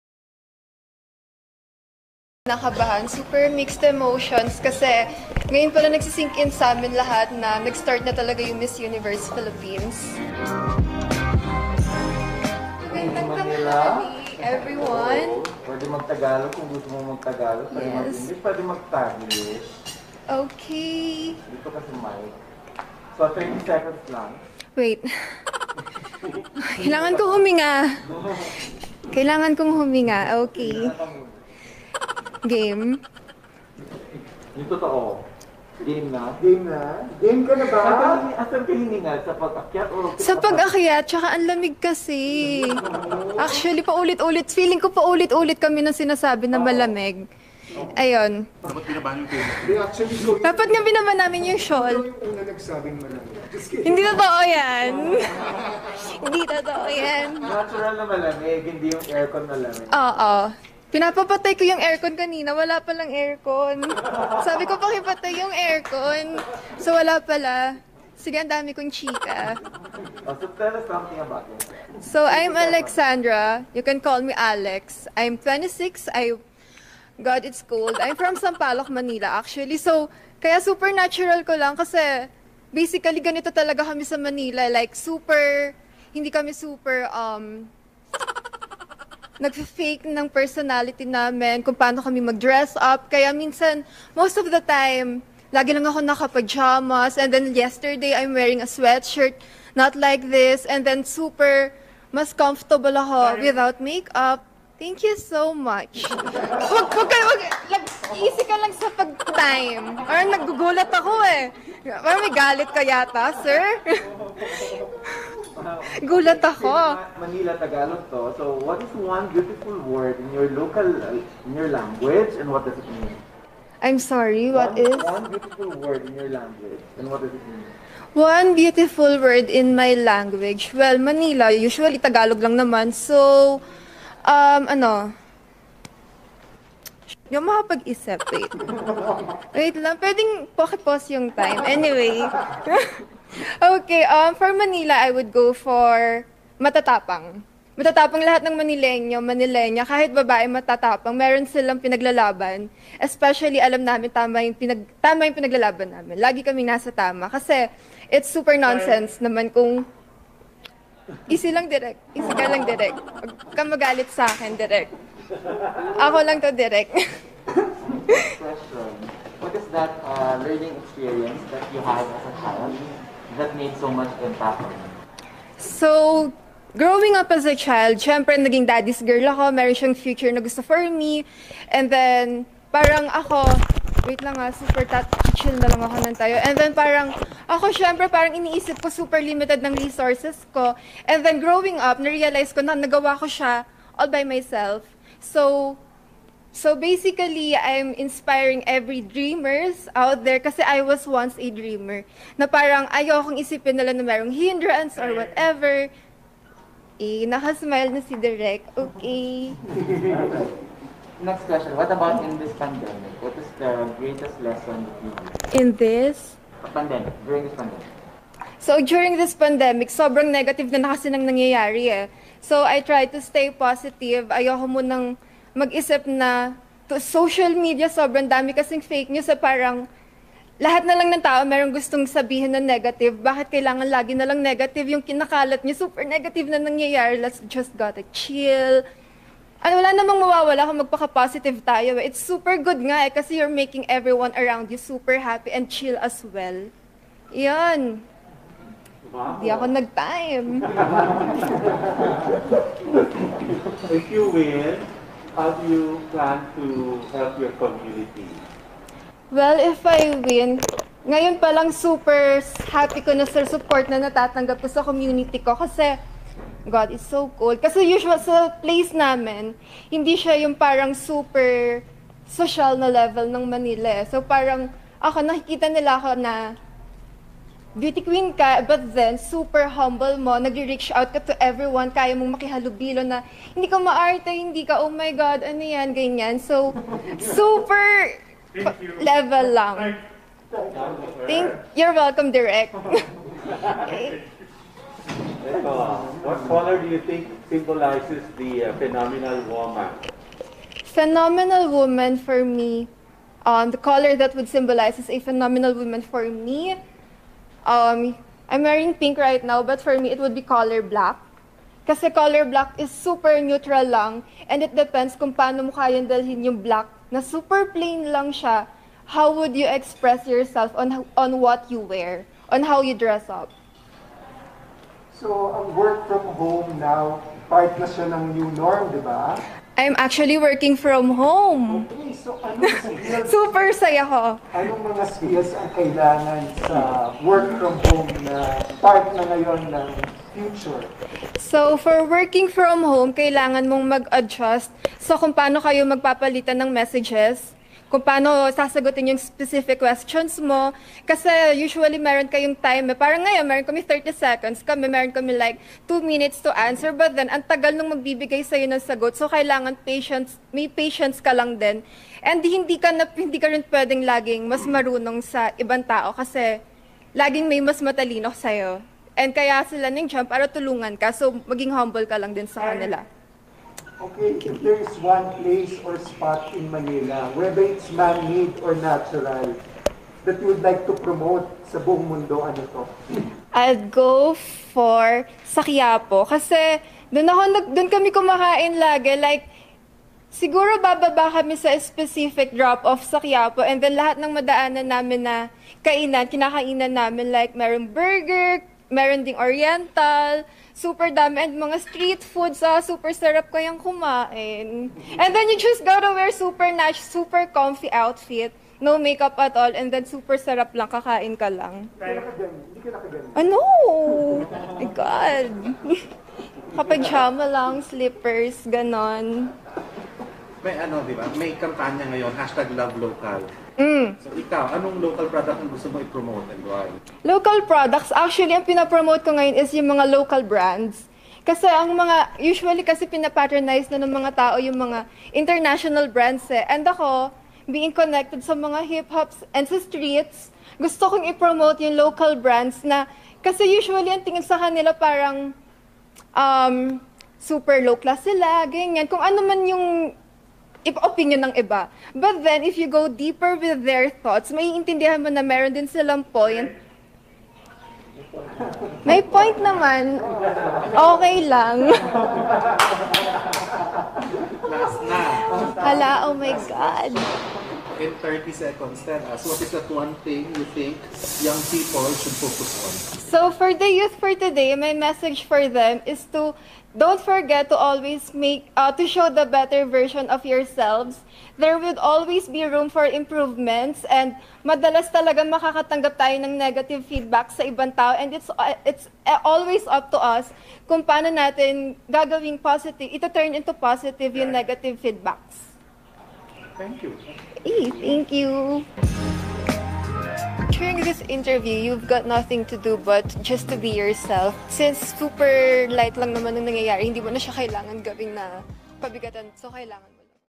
MANILA REPRESENTATIVE ANG NAGDALA SA PRELIMINARY NG MISS UNIVERSE PHILIPPINES/MUP PRELIMINARY
Super chill lang si ate gurl eh haha lab it ♥